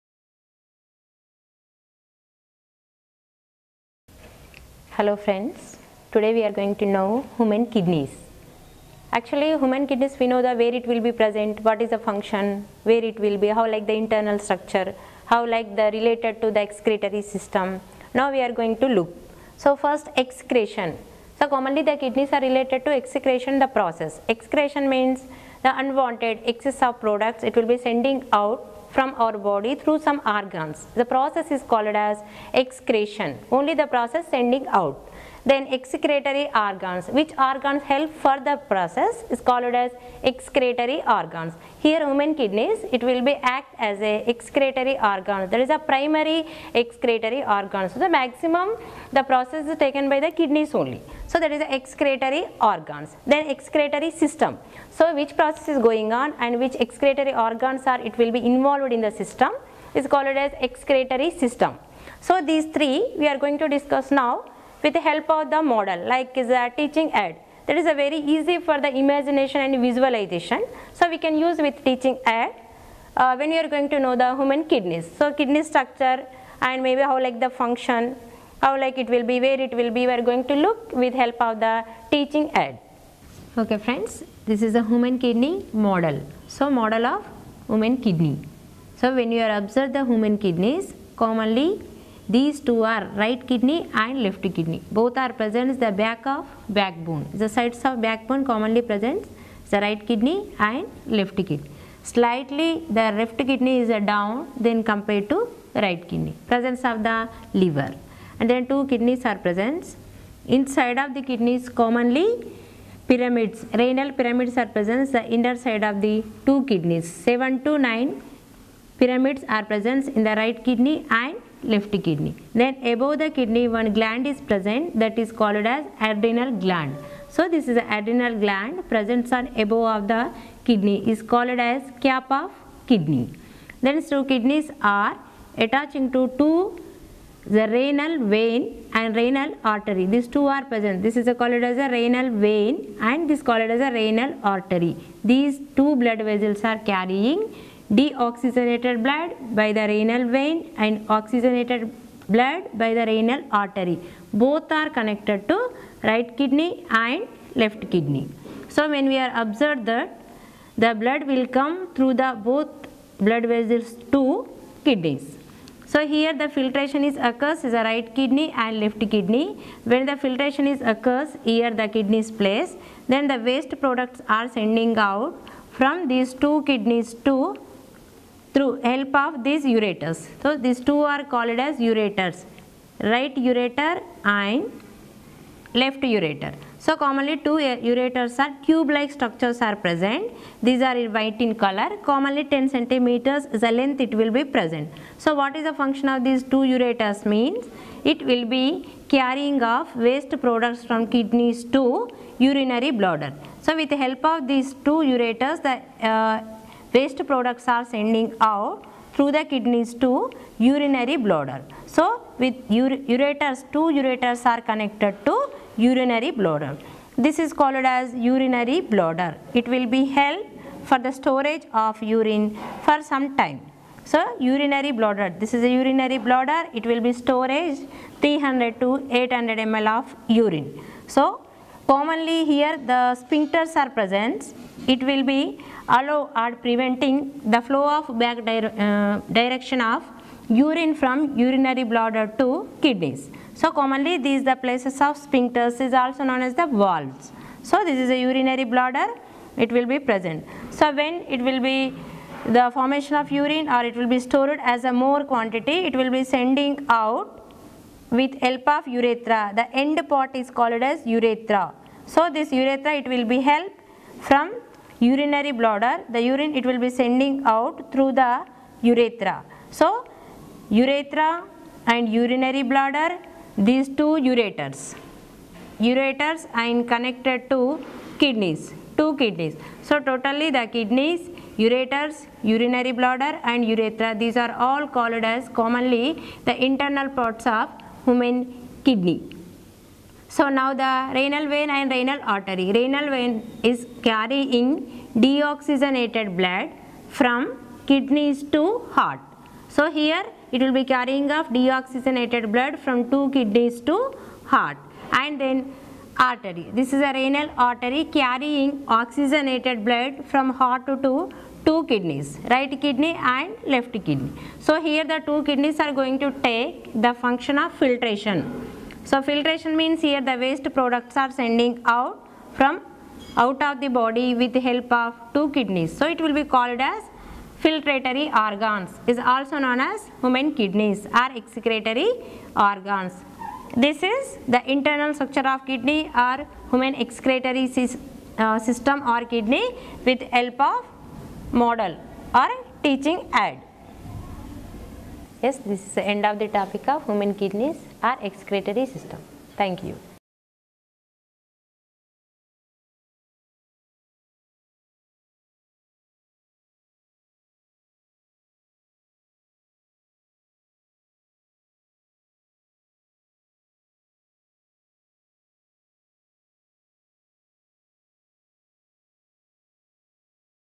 Human kidney (Biology class 7,8,9,10)
This video talks about the hman excretory system/ human kidneys